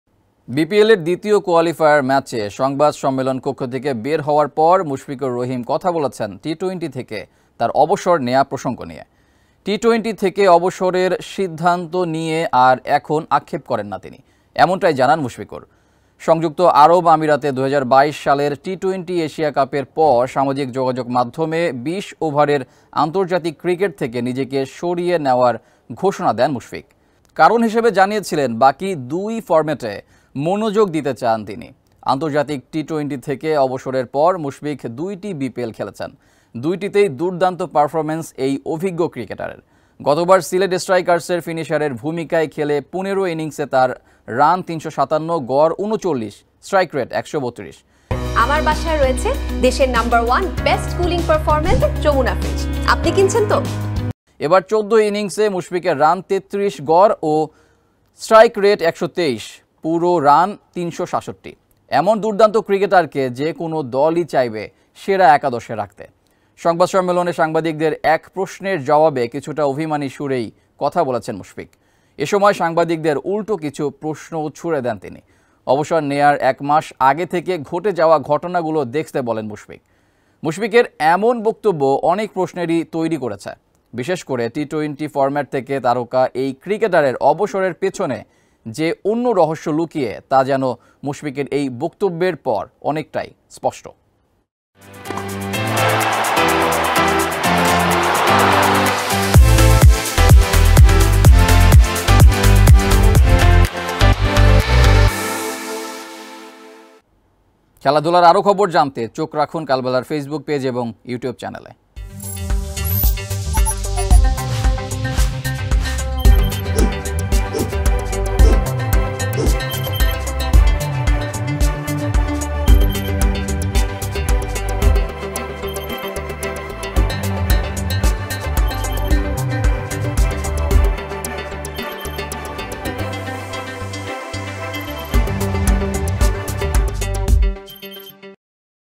অবসর নিতে বাধ্য হন মুশফিক | Mushfiqur Rahim | BPL | Kalbela
বিপিএলের দ্বিতীয় কোয়ালিফায়ার ম্যাচে সংবাদ সম্মেলন কক্ষ থেকে বের হওয়ার পর মুশফিকুর রহিম কথা বলেছেন টি-টোয়েন্টি থেকে তার অবসর নেয়া প্রসঙ্গ নিয়ে।  টি-টোয়েন্টি থেকে অবসরের সিদ্ধান্ত নিয়ে এখন আর আক্ষেপ করেন নেই তার।  এমনটাই জানান মুশফিকুর। সংযুক্ত আরব আমিরাতে ২০২২ সালের টি-টোয়েন্টি এশিয়া কাপের পর সামাজিক যোগাযোগমাধ্যমে ২০ ওভারের আন্তর্জাতিক ক্রিকেট থেকে নিজেকে সরিয়ে নেওয়ার ঘোষণা দেন মুশফিক। অবসর নিতে বাধ্য হন মুশফিক | Mushfiqur Rahim | BPL | Kalbela